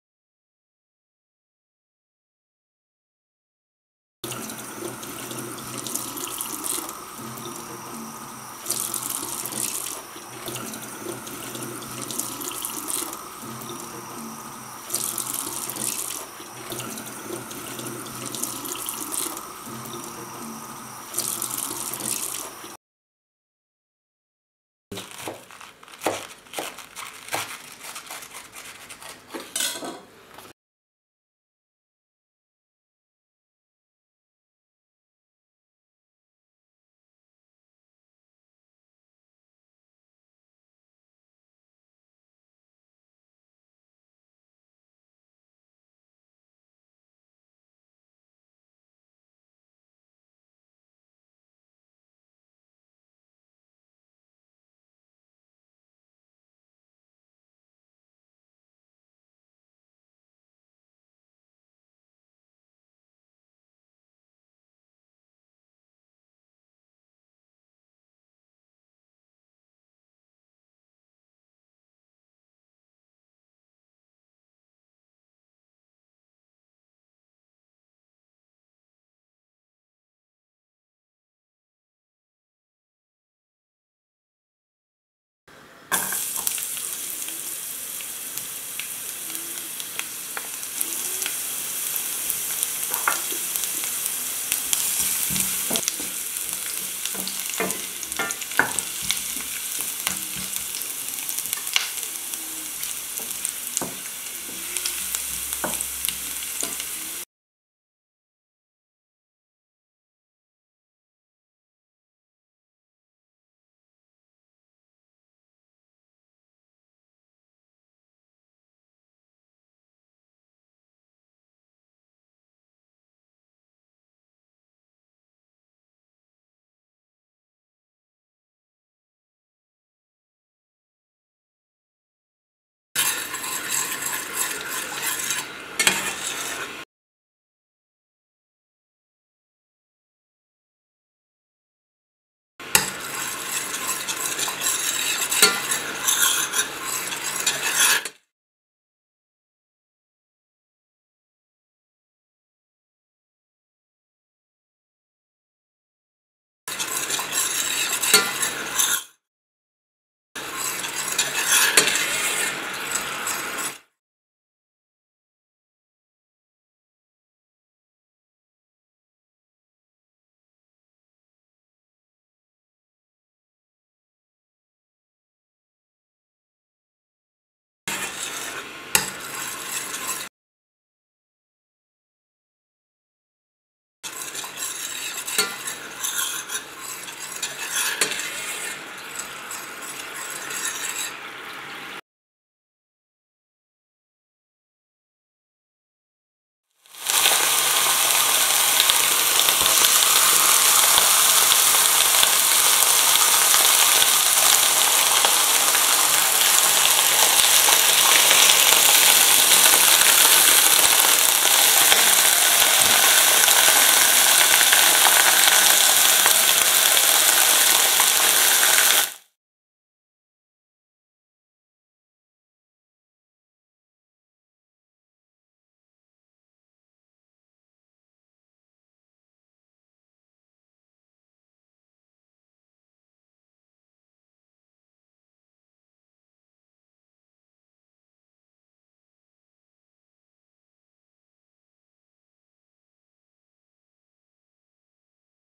Pak Choi blanched and fried with oyster sauce + crispy garlic - a quick and healthy Pak Choi recipe.
How to prepare Pak Choi - blanched and fried with oyster sauce + crispy garlic - a quick and healthy Pak Choi recipe.Served with oyster sauce. A vegetarian - quick and easy Pak Choi recipe with Asian sauce. Here you will find the list of ingredients for the Pak Choi recipe

3    Pack Choi
3    cloves of garlic

You need for the oyster sauce
4 tbsp     of water
3 tbsp     of water mixed with cornstarch - to bind the oyster sauce
                how to make the mixture - mix 8 tbsp of water with 2 tbsp of cornstarch - done
2 tbsp     soy sauce
2 tbsp     oyster sauce
1 tsp       sugar
and some freshly ground black pepper
some oil for frying
I used rapeseed oil - you can use an oil of your choice